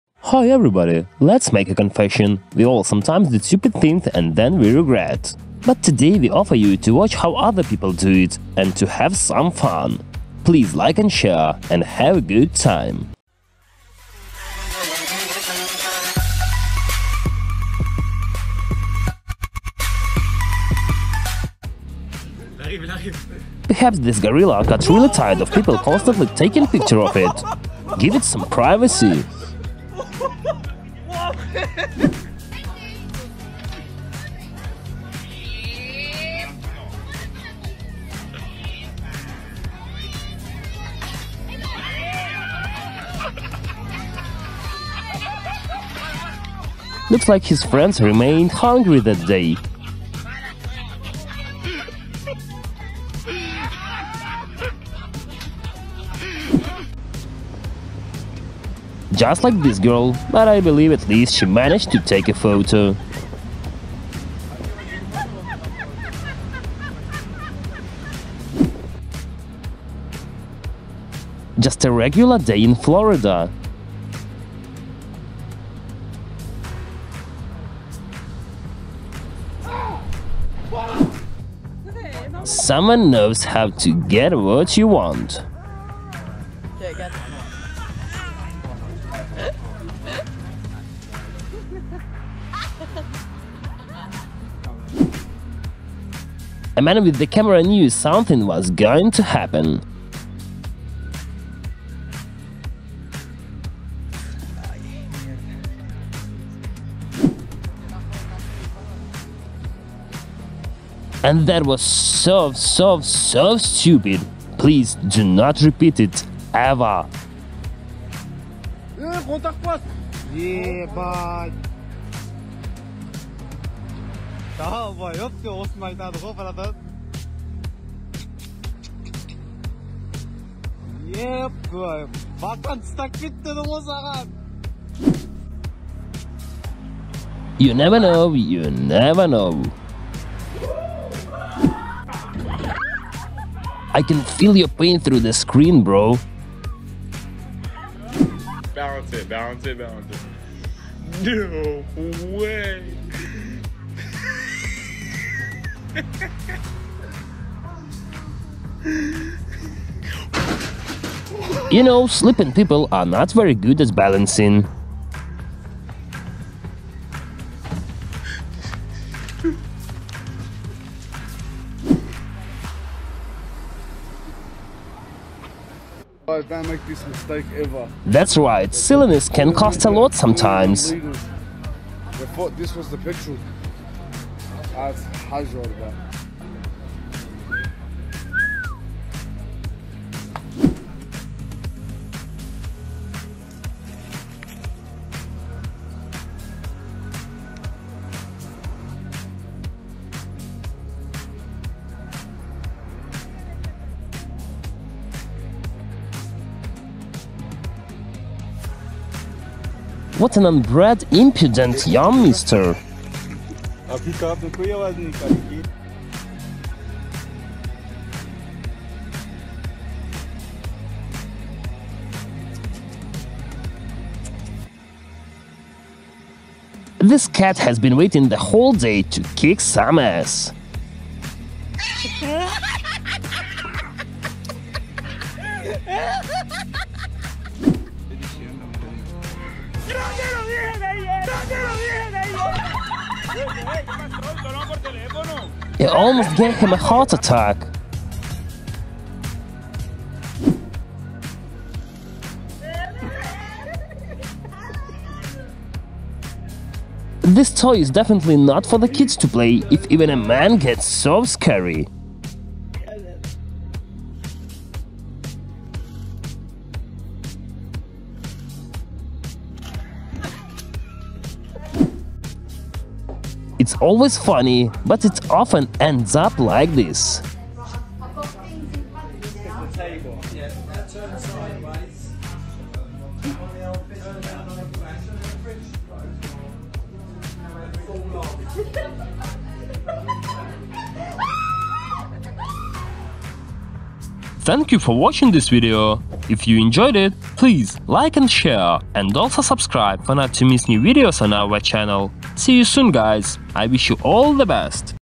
16 Instant Regret Moments Caught on Camera
Let's make a confession - we all sometimes make mistakes and then we regret. But today we offer you to watch how other people do it. Watch people instantly regret their silly actions and have fun!